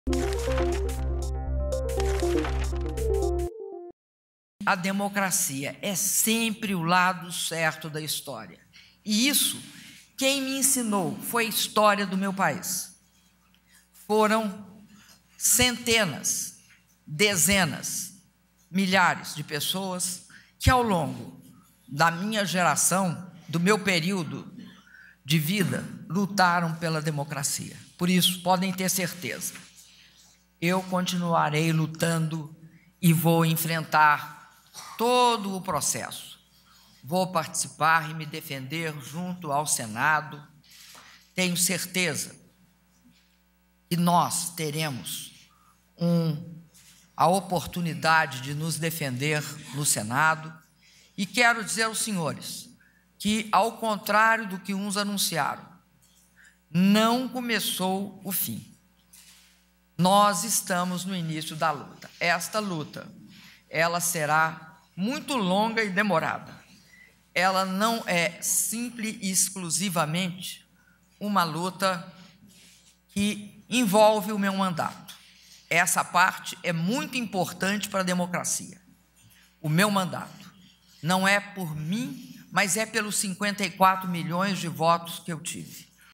Presidenta diz que continuará lutando por democracia
EM DESTAQUE - 18.04.16: Em sua primeira declaração após a abertura do processo de impeachment, a presidenta Dilma Rousseff afirmou que vai continuar lutando pela democracia, por seu mandato e pelo 54 milhões de eleitores que votaram nela.